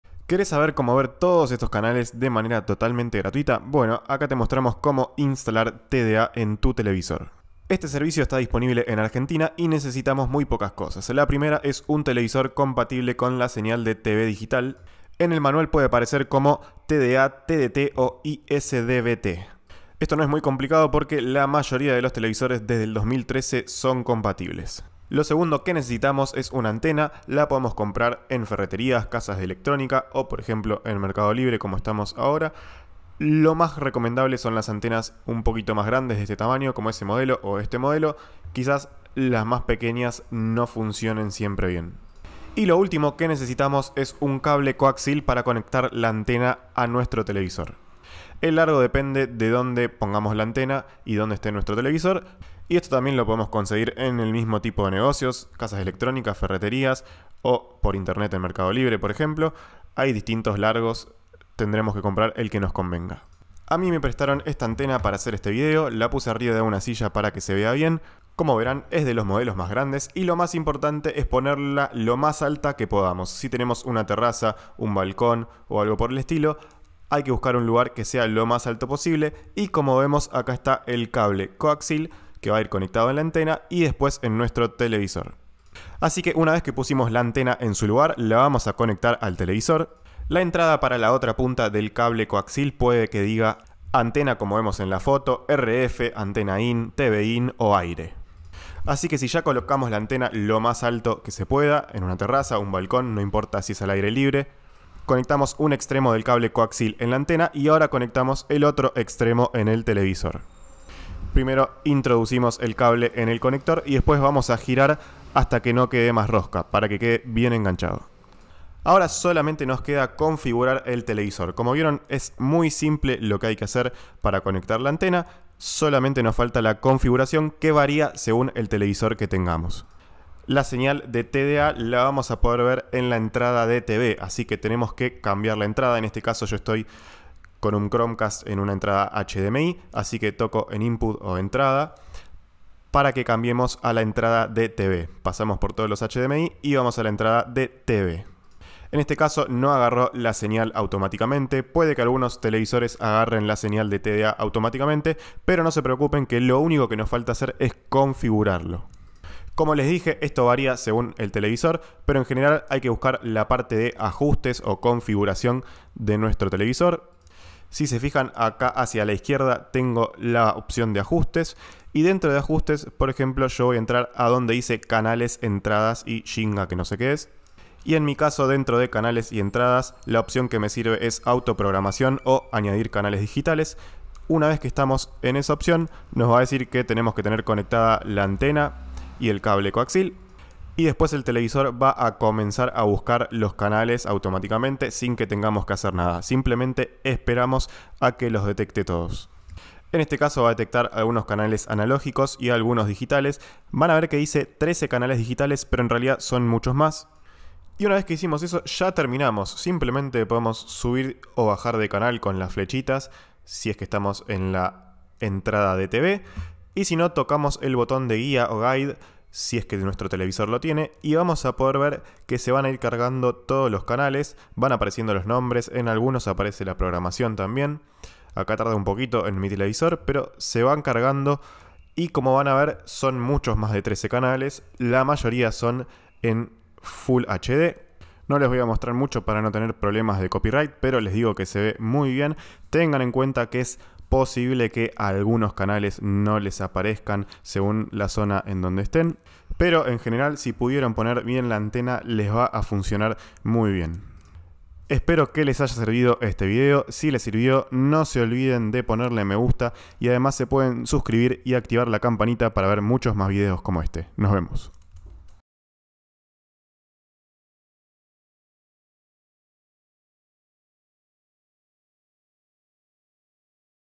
CÓMO INSTALAR Y CONFIGURAR ANTENA TDA | VER CANALES TDA ARGENTINA 📺
Te mostramos cómo instalar la antena TDA argentina y cómo configurar y orientar todo para ver los canales de TDA. Seguí el tutorial paso a paso para hacerlo vos mismo.

En el video te mostramos todo lo que necesitás (TV compatible con TDA. cable coaxil y una antena) y te mostramos cómo conectar todo para disfrutar de los canales del servicio.